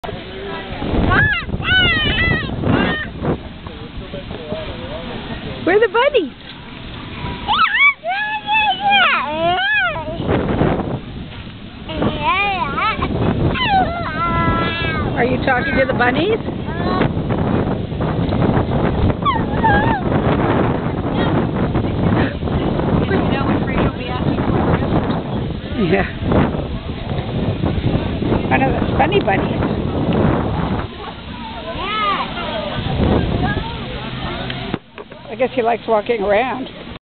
Fraser talks to the bunnies
Fraser talked to the bunnies about how much he likes them (smaller than he is a good thing)